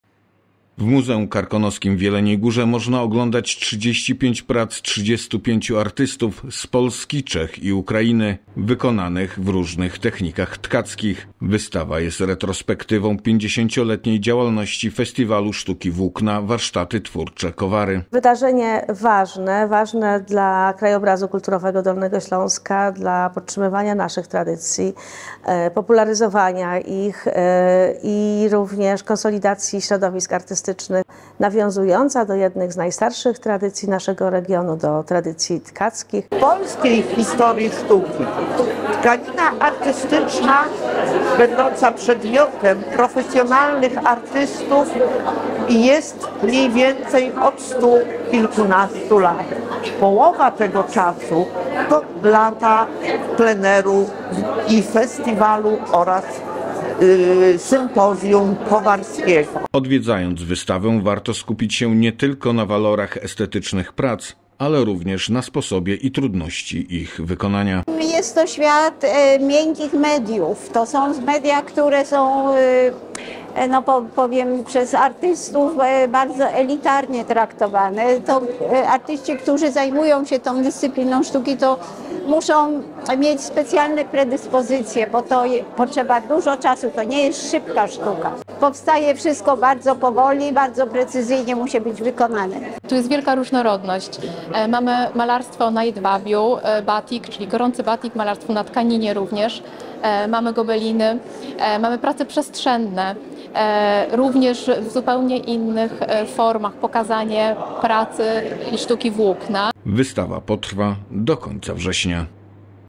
Sztuka włókna w Muzeum Karkonoskim
W Muzeum Karkonoskim w Jeleniej Górze można oglądać 35 prac 35 artystów z Polski, Czech i Ukrainy wykonanych w różnych technikach tkackich, wystawa jest retrospektywą 50-letniej działalności Festiwalu Sztuki Włókna Warsztaty Twórcze Kowary, w ekspozycji zobaczymy prace uczestników festiwalu w latach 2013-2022.